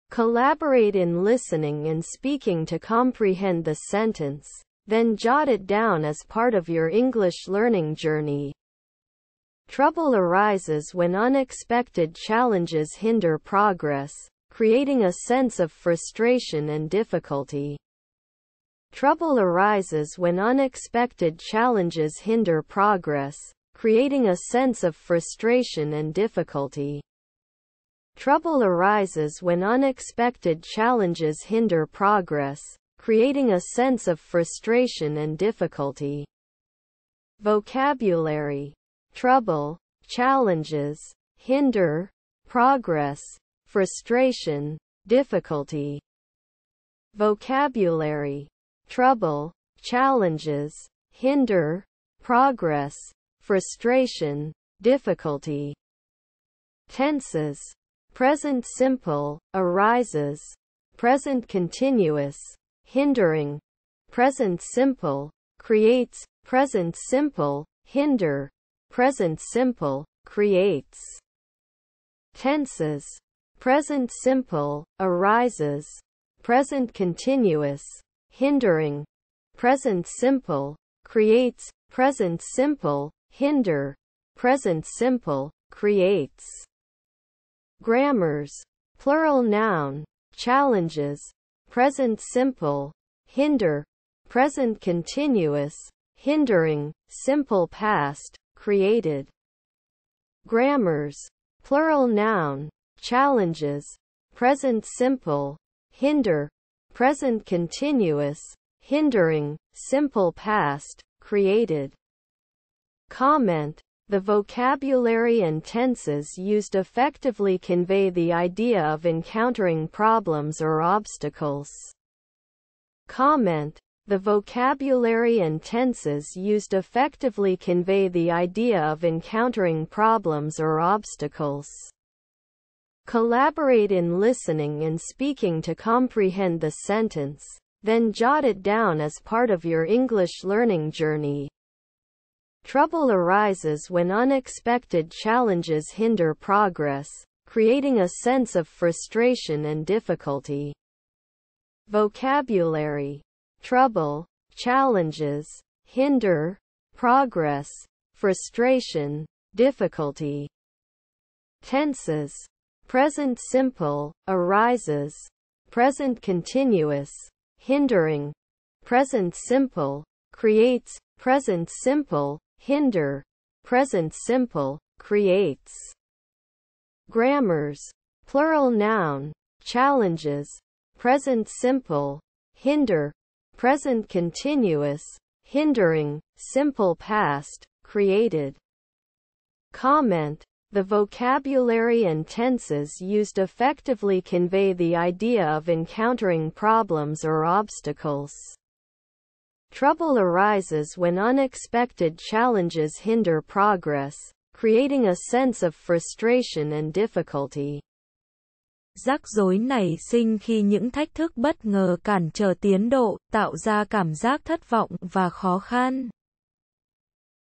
Trouble arises when unexpected challenges hinder progress, creating a sense of frustration and diffi
Collaborate in listening and speaking to comprehend the sentence, then jot it down as part of your English learning journey.

Trouble arises when unexpected challenges hinder progress, creating a sense of frustration and difficulty.

Vocabulary: trouble, challenges, hinder, progress, frustration, difficulty.

Tenses: present simple (arises), present continuous (hindering), present simple (creates), present simple (hinder), present simple (creates).

Grammars: plural noun (challenges), present simple (hinder), present continuous (hindering), simple past (created).

Comment: The vocabulary and tenses used effectively convey the idea of encountering problems or obstacles.